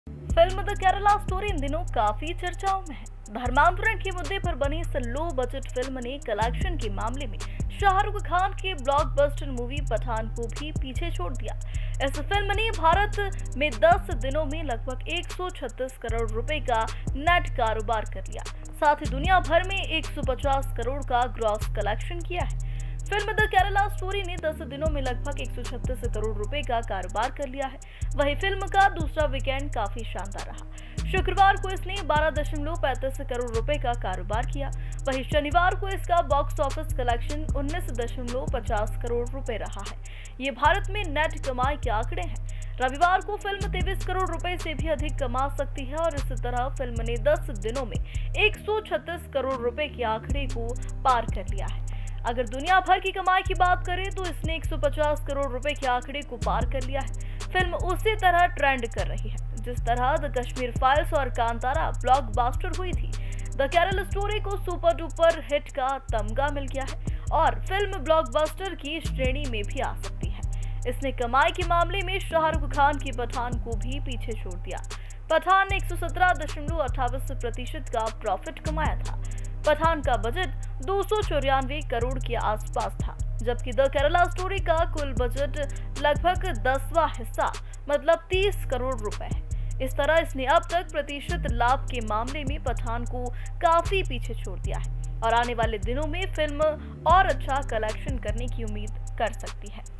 The Kerala Story ने Pathan को पीछे छोड़ा, कलेक्शन में बनाया नया कीर्तिमान | ACN BHARAT
ACN Bharat
ACN भारत
Hindi News
MP News
Madhya Pradesh News
TV News
Political News
Politician News
RJ News
Rajasthan News